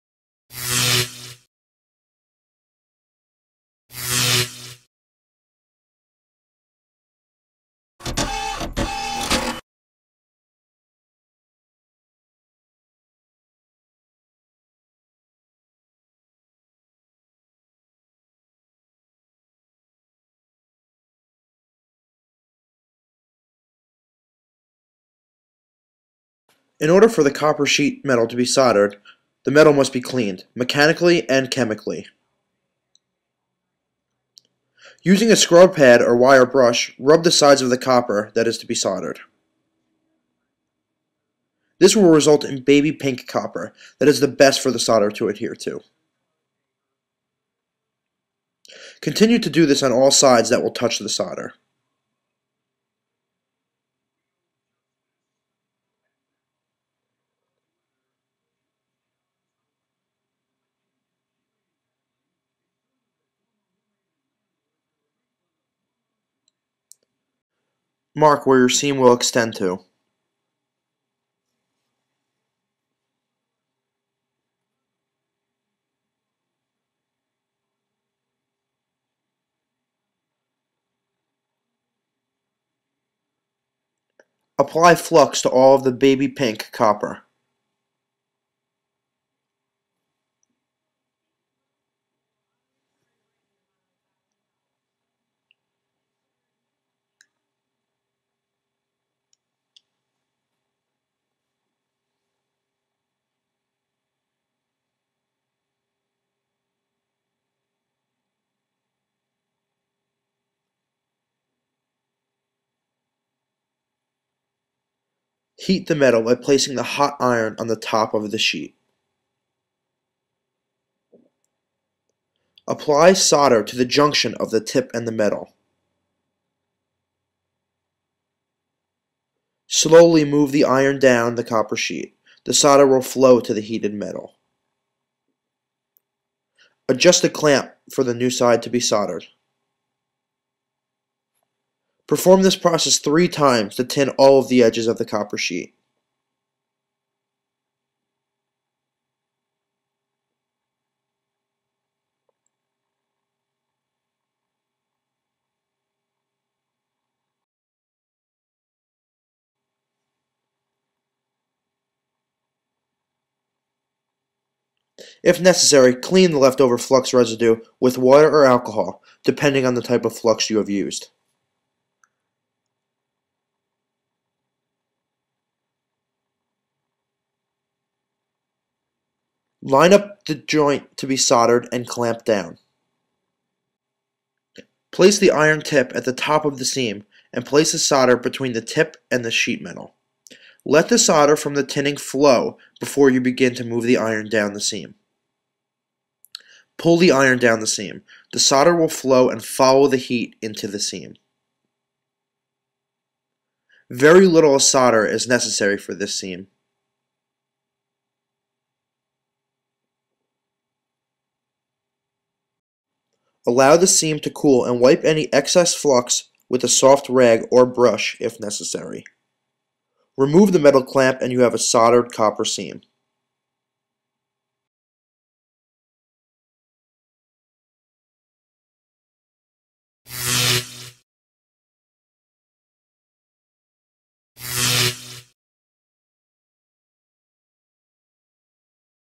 Hexacon - Soldering a Copper Seam
How to properly solder a copper seam using a Hexacon Electric SI-P300

Hexacon Electric Company is a proud manufacturer of solder tools and accessories located in Roselle Park, NJ since 1932.

Visit us on IG @hexaconelectriccompany